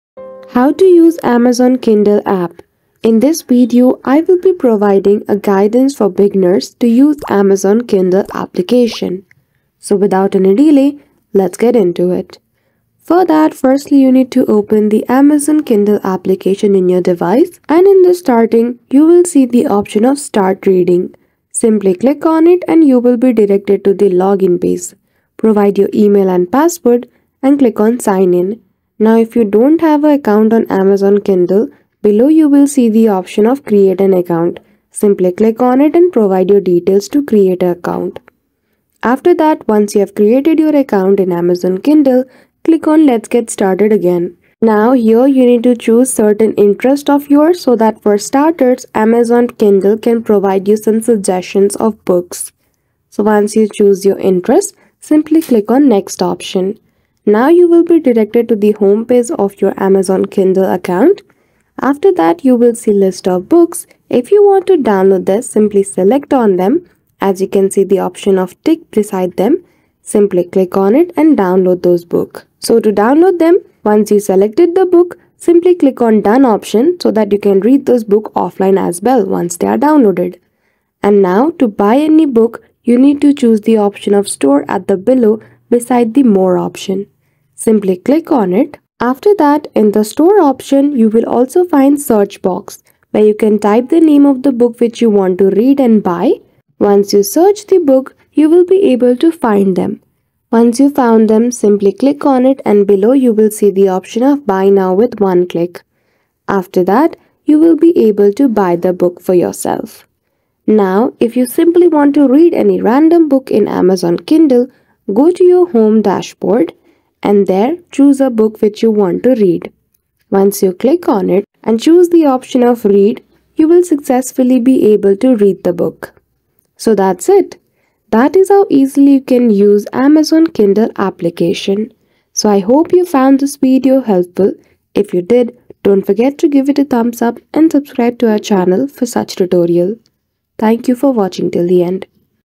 How to Use Amazon Kindle App 2024
New to the Amazon Kindle app and want to learn how to use it in 2024? This easy guide provides step-by-step instructions on how to navigate and use the Amazon Kindle app for reading eBooks and listening to audiobooks. Follow these straightforward steps to enhance your reading experience.

In this tutorial:
0:00 - Introduction
0:15 - How to Use Amazon Kindle App in 2024 (EASY)

Learn how to effortlessly use the Amazon Kindle app in 2024 with this simple guide. This video covers the entire process, from downloading and setting up the app to exploring features like purchasing eBooks, adjusting reading settings, and listening to audiobooks.

Improve your reading experience by following this guide to use the Amazon Kindle app!